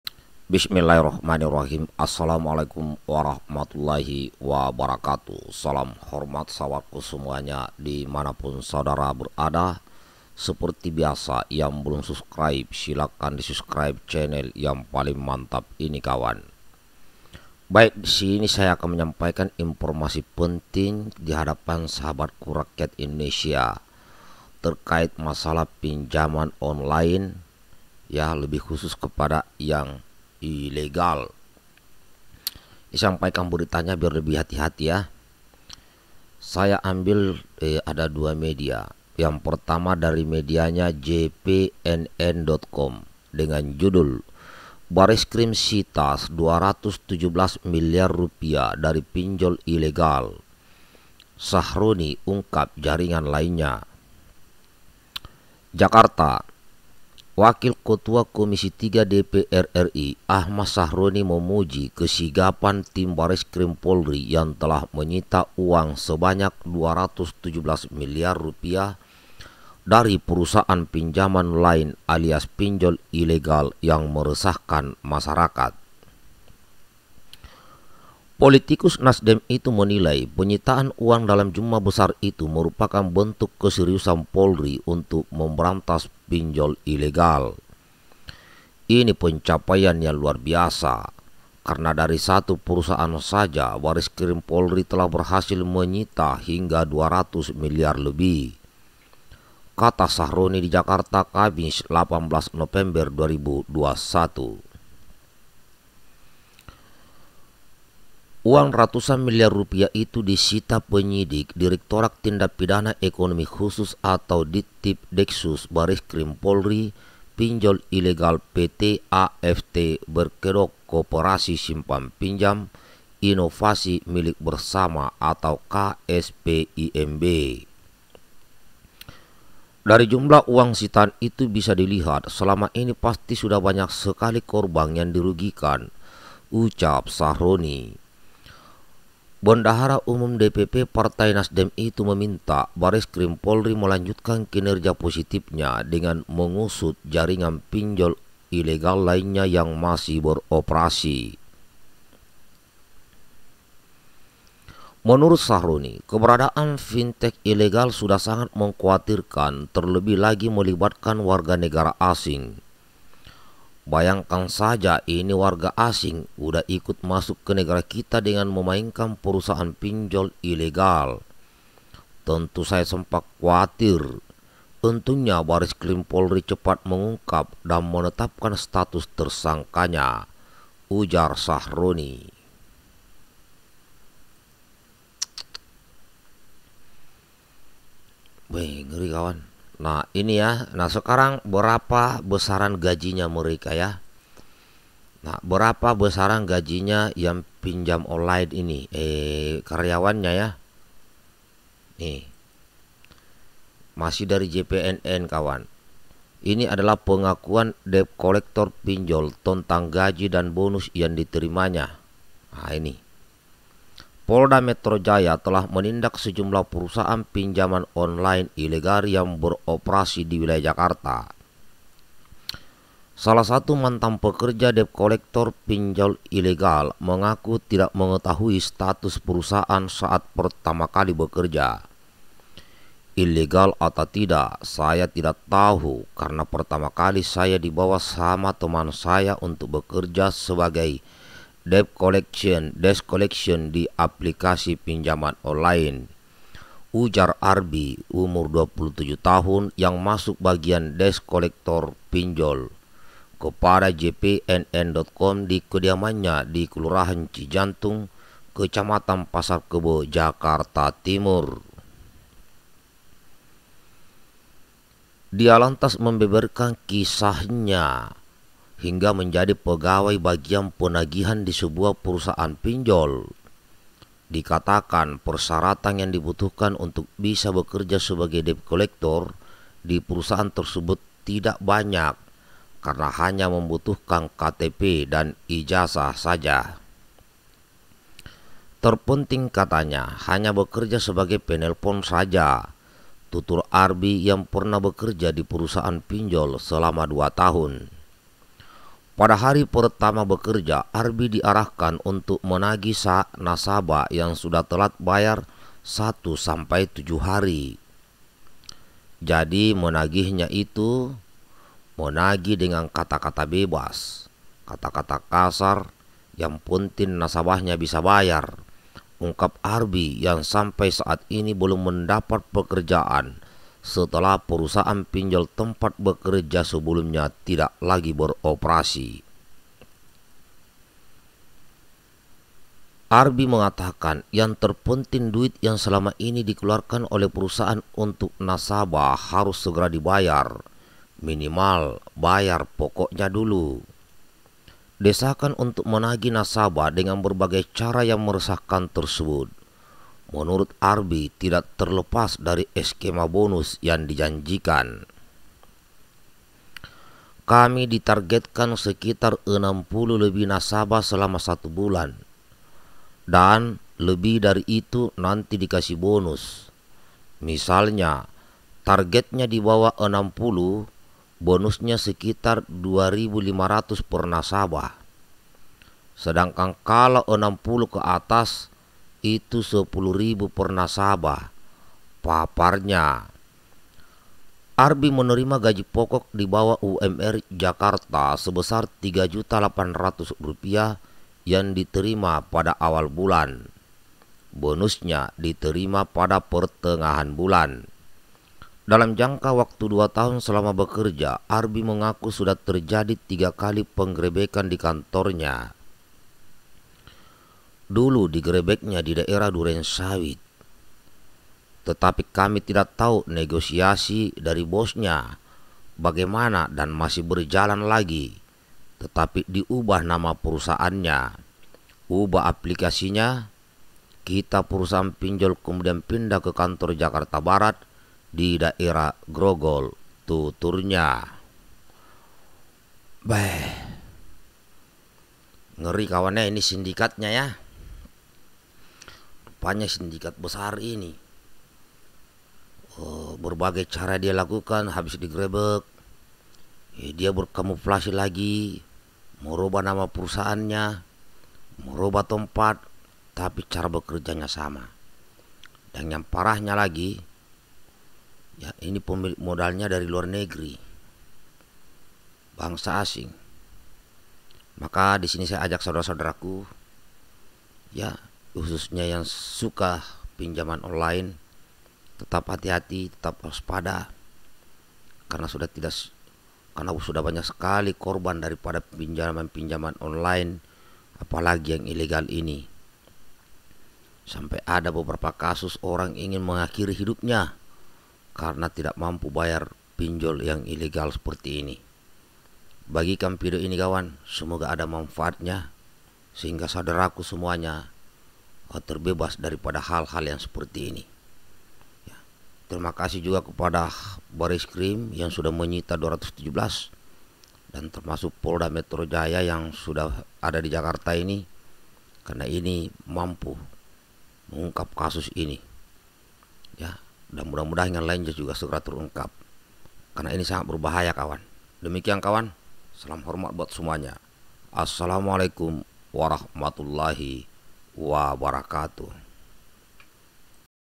SITA 217 MILYAR PINJOL ILEGAL. BESARAN GAJI KARYAWAN PINJOL. MPS : WASPADA PINJOL ILEGAL DI MEDSOS
BANTU VIRALKAN...

Aamiin.

TTD